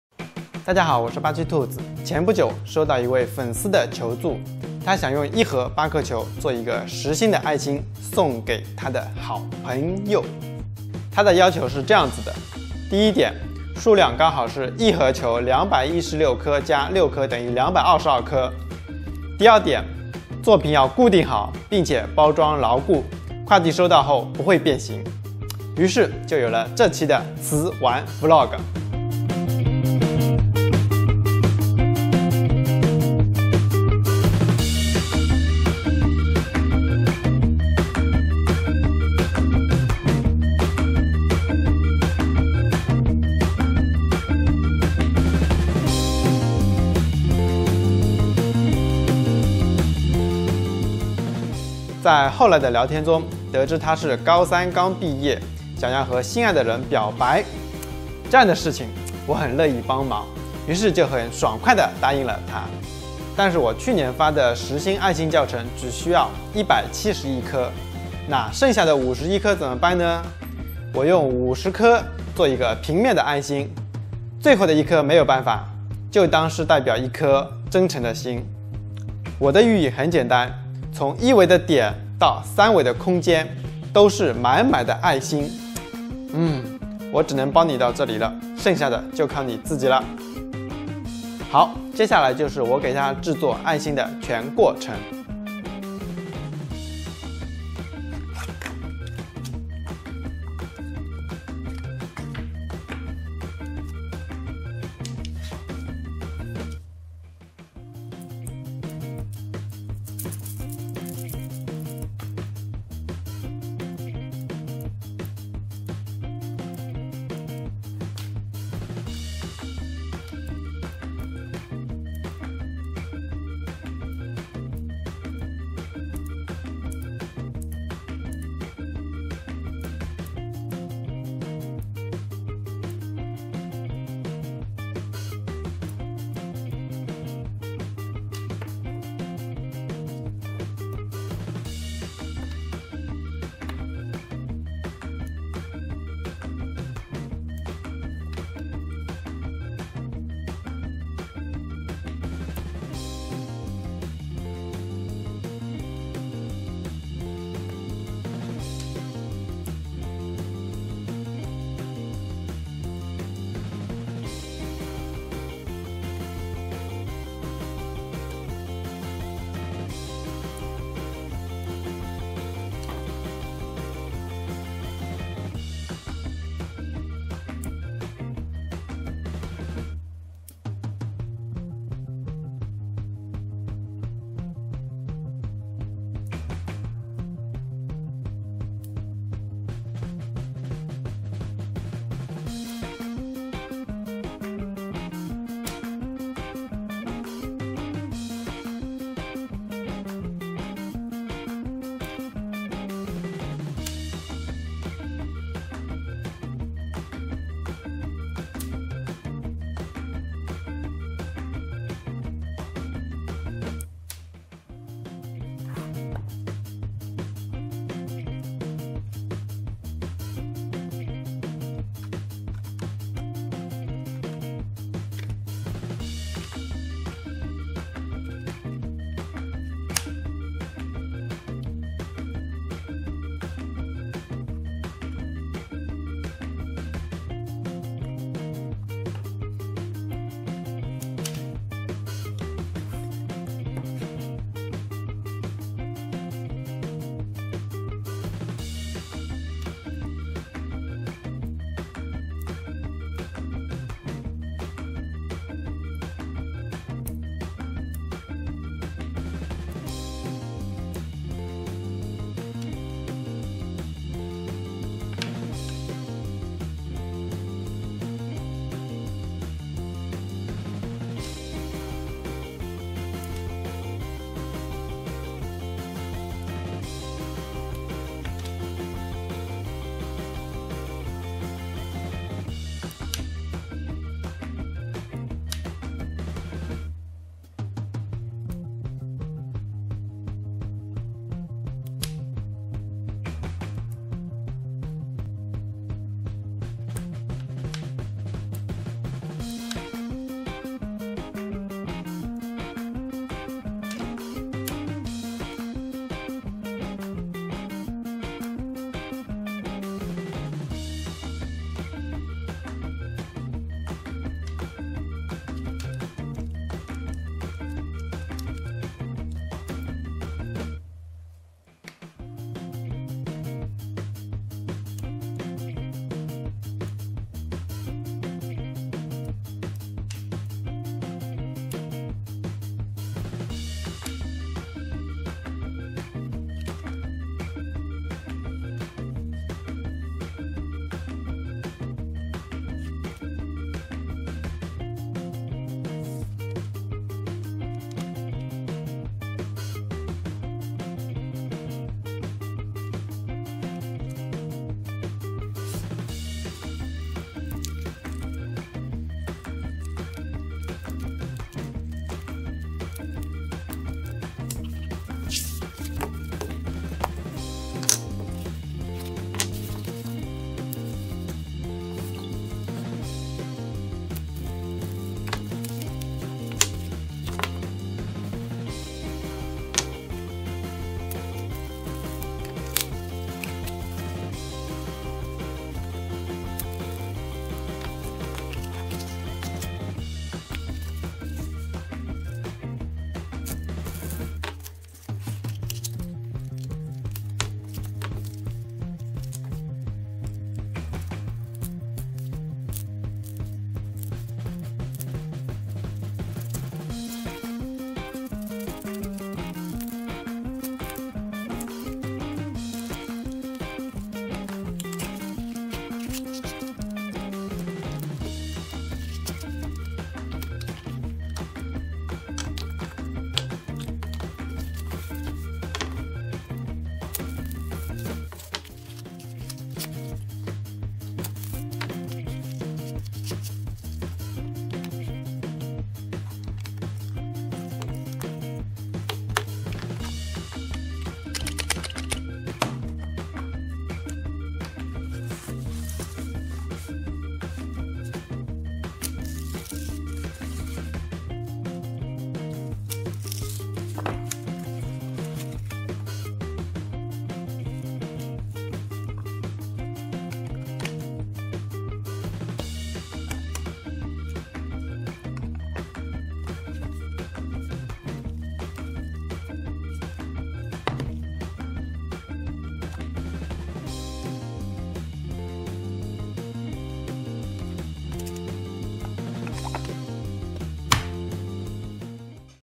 How to make a love gift for first love
From a graduate fan's help, he hopes that I can help him to make love works, and package it, so I have this video, everyone bless him.